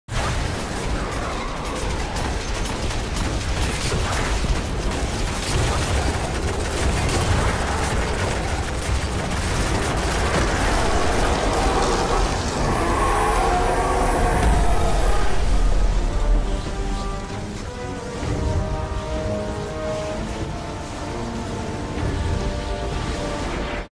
At The Buzzer
Hard Mode Warpriest kill at the last moment on our 2nd glyph sequence!